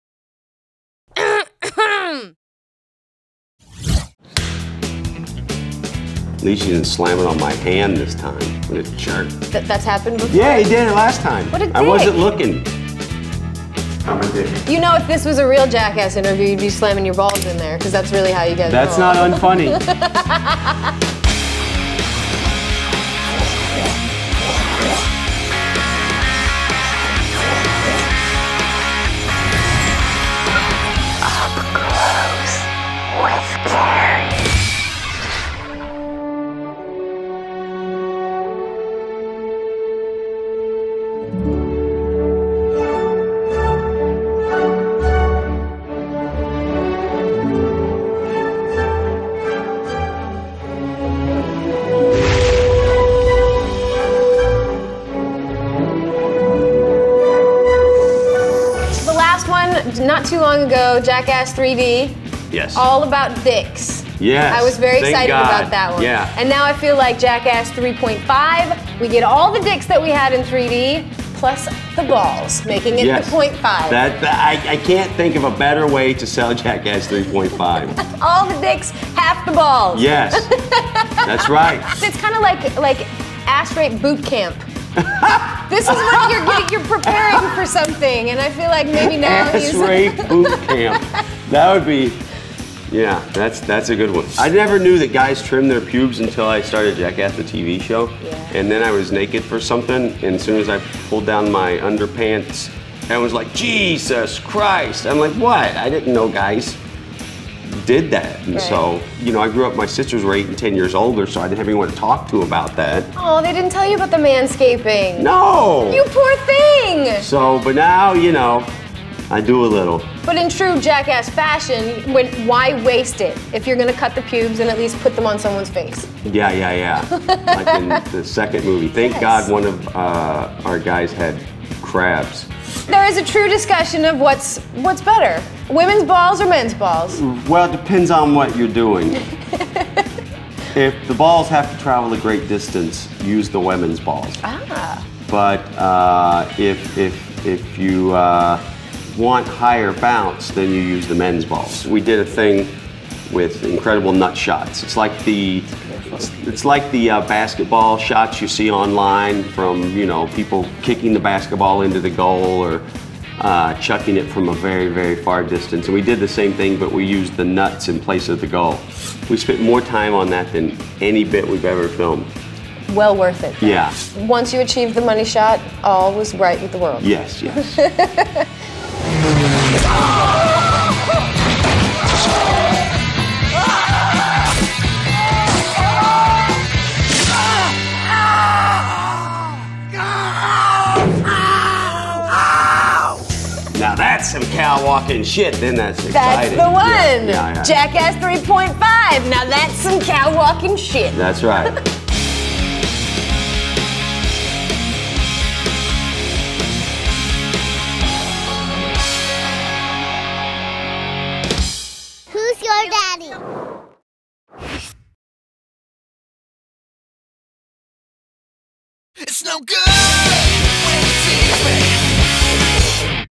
Johnny Knoxville & Bam Margera on Jackass 3D
Johnny Knoxville, Bam Margera, & Steve-O on Jackass 3D